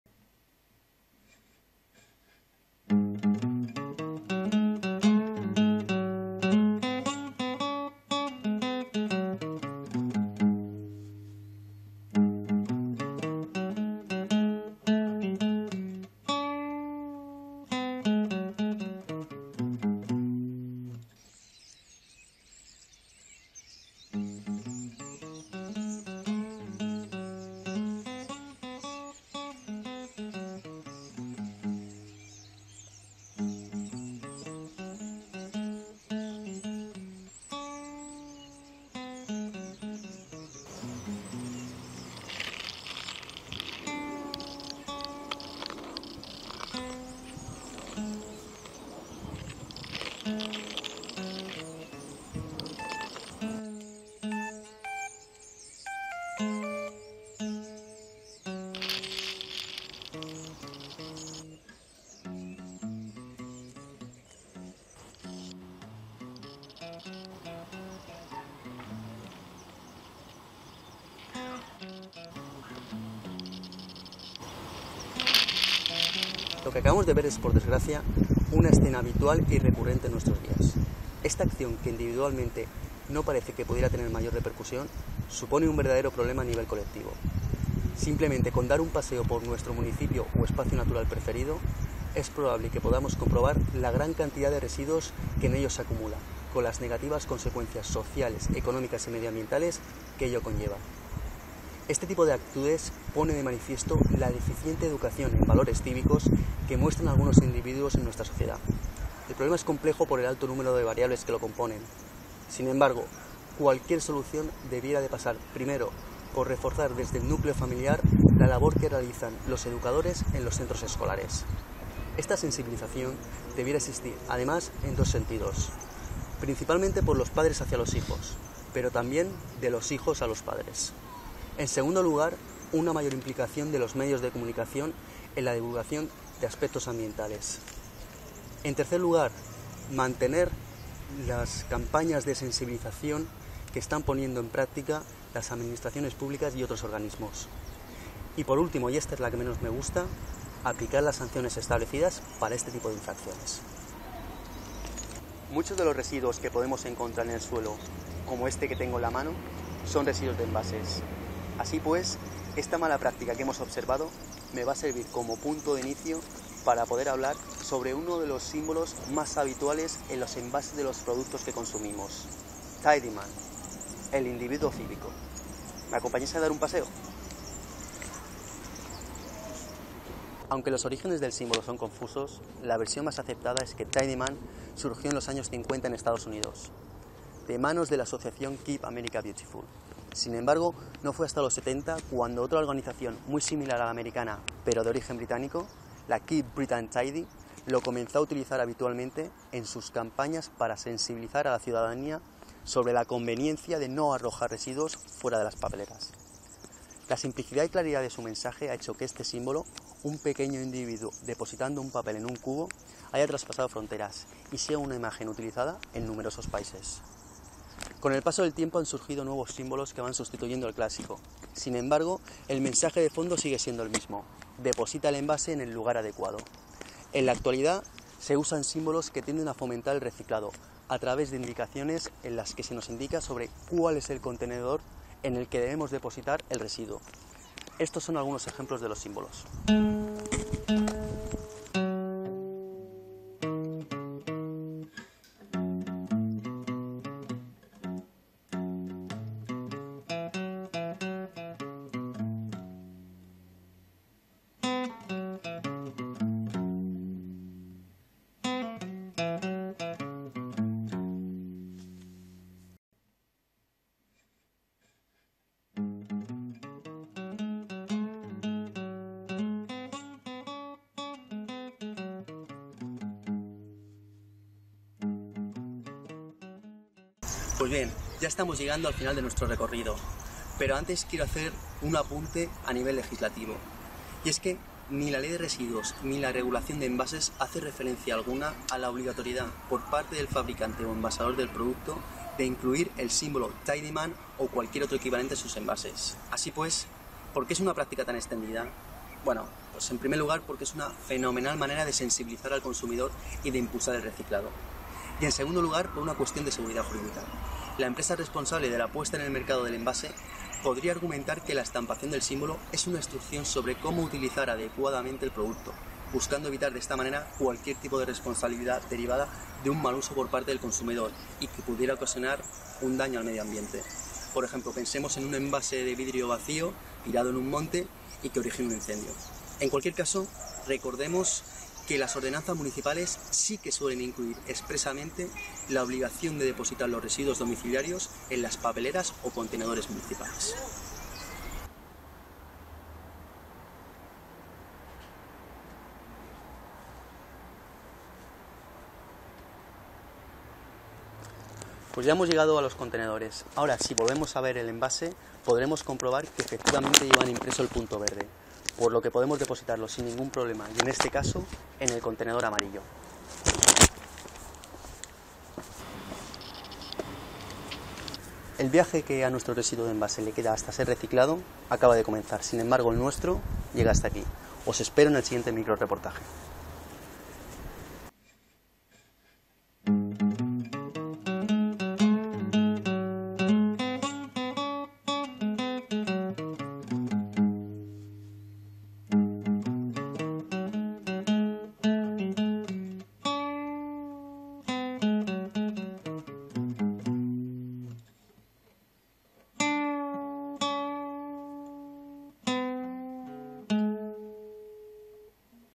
µSostenible. El etiquetado de los productos: Tidyman
Tidyman es uno de los símbolos más habituales en los envases de los productos que consumimos. ¿Quieres saber algo más de él?

Quienquiera dejar un comentario puede hacerlo aquí o en la entrada del blog (si lo hacéis en el blog podría contestar con mayor rapidez).